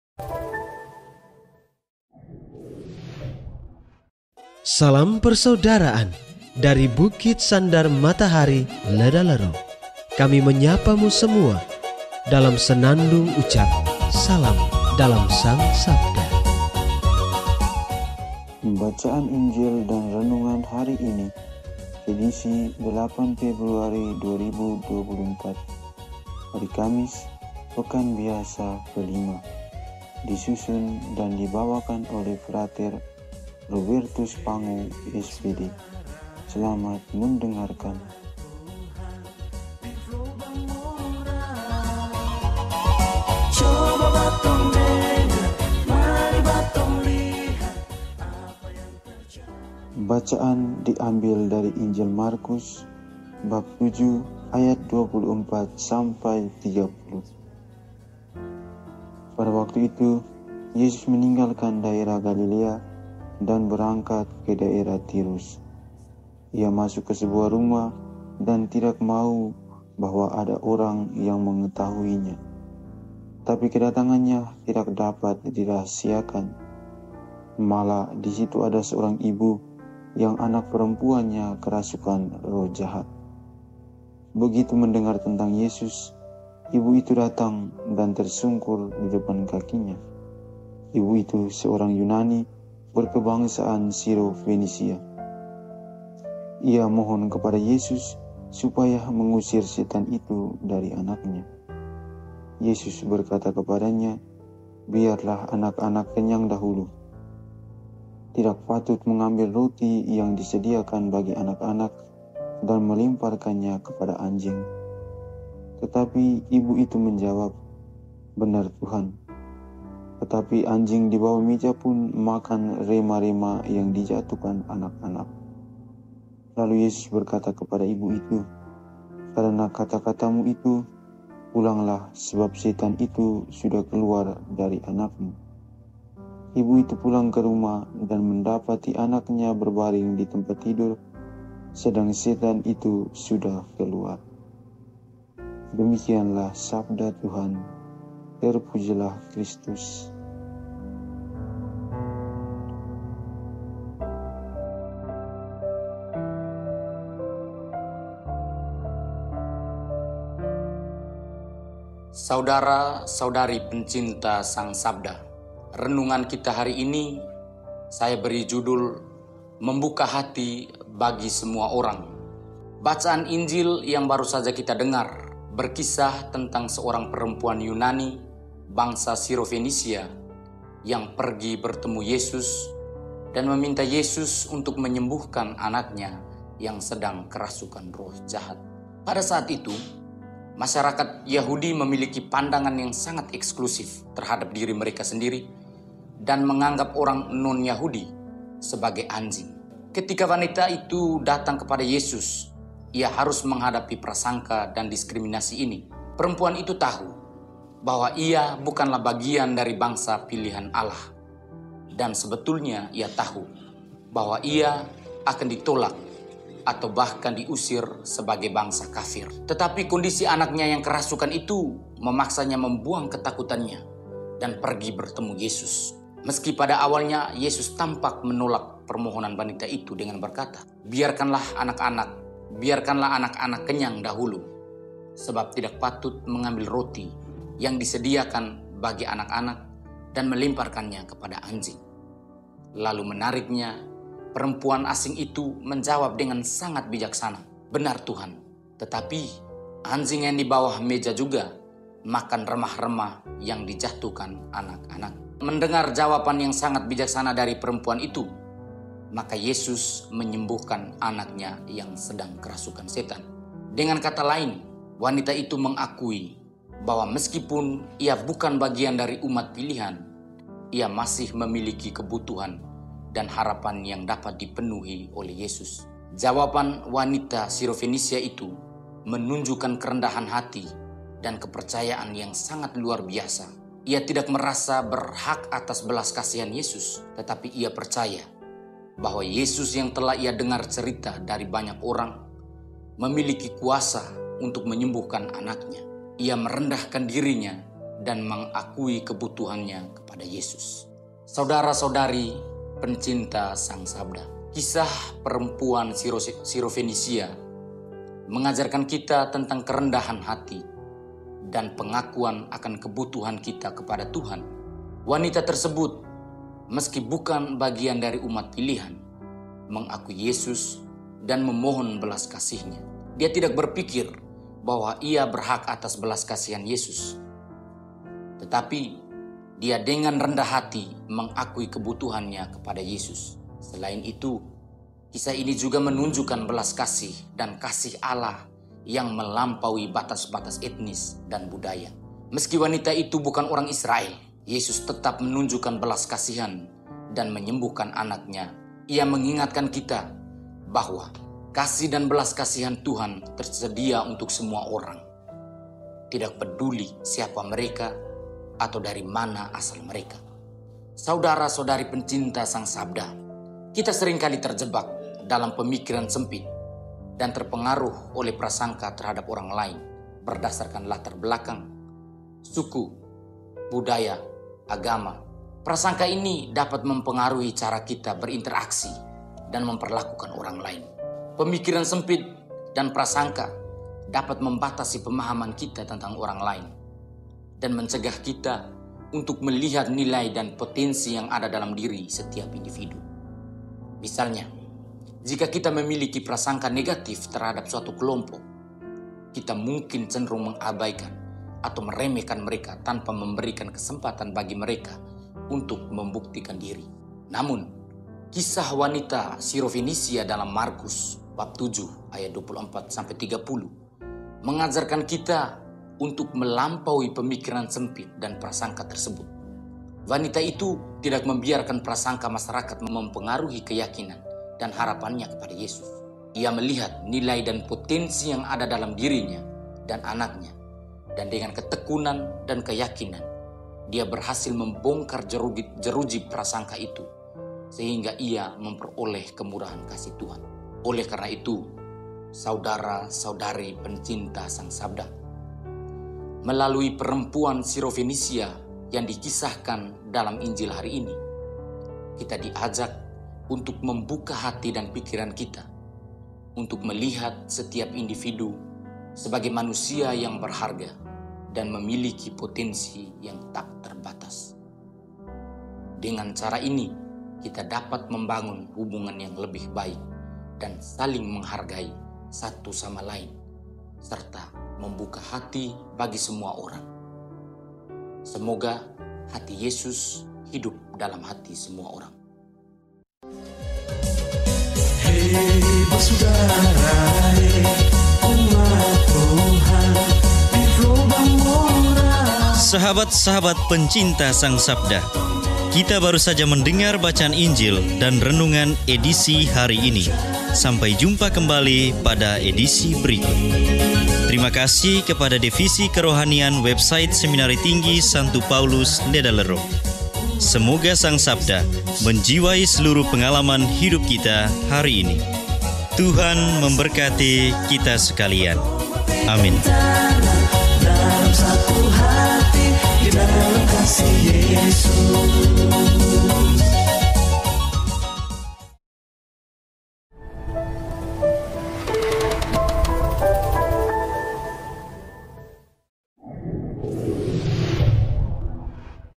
MEMBUKA HATI BAGI SEMUA ORANG - (Kamis, 8 Februari 2024) Renungan Harian Frater Ledalero
Renungan disusun oleh: Fr. Robertus Pangu, SVD